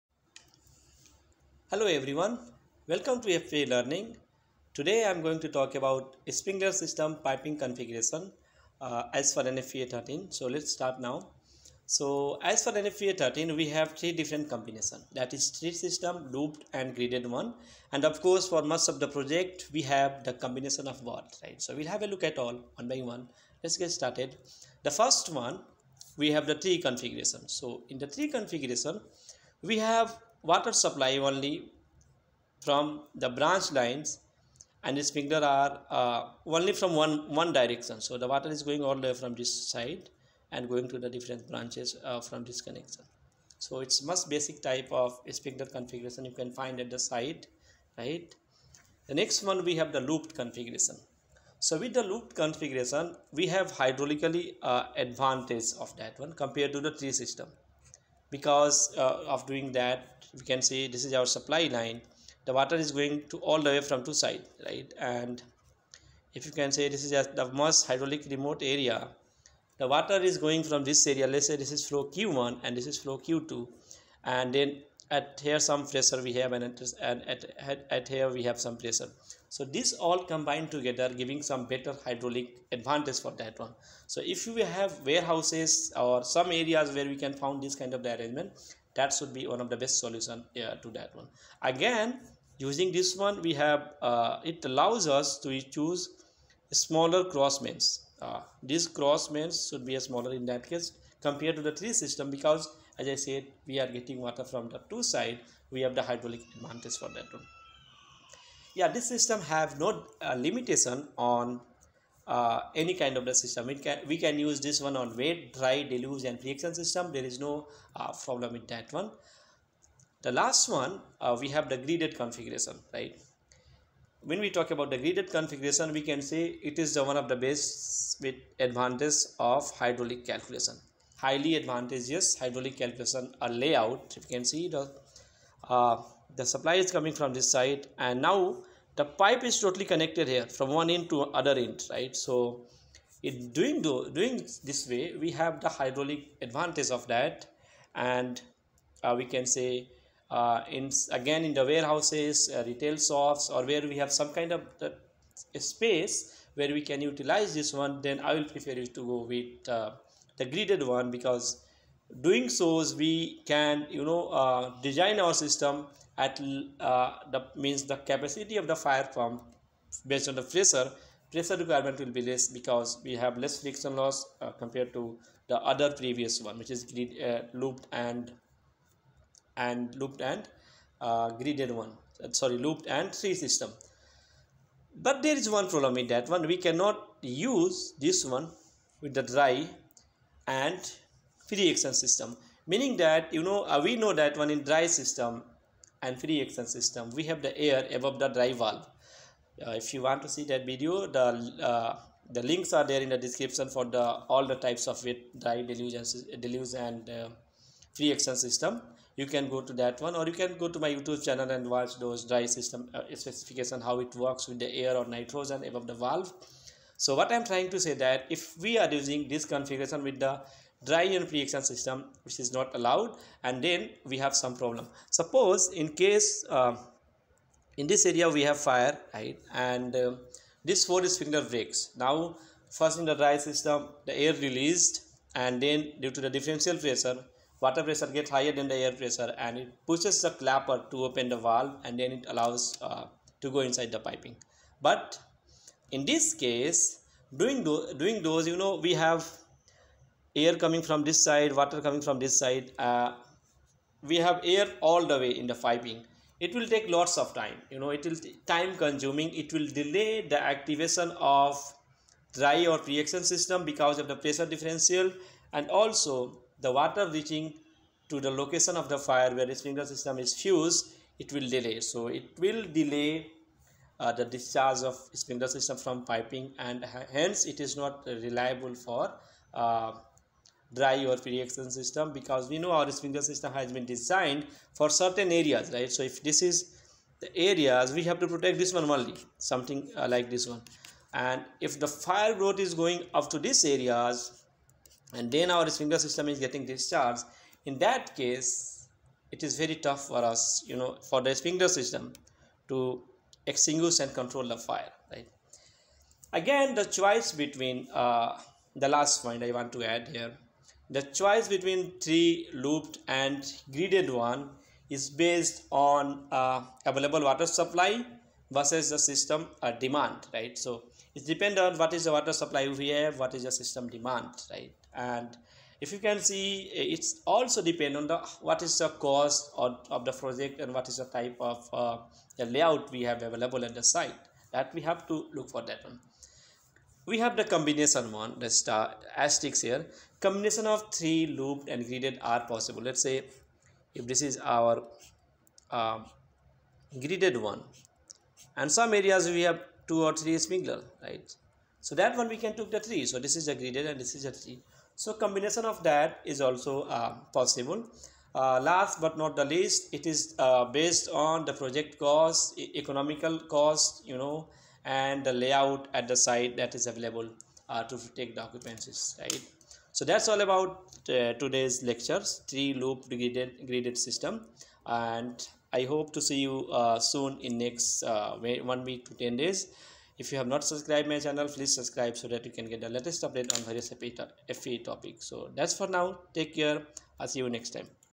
Sprinkler System Piping Configuration
This video describe about sprinkler system piping configuration as per NFPA 13 .
Tree
Looped
Gridded and combination

Wet Pipe Sprinkler System

Dry Pipe Sprinkler System

Deluge Valve - Part 1 : Wet pilot actuation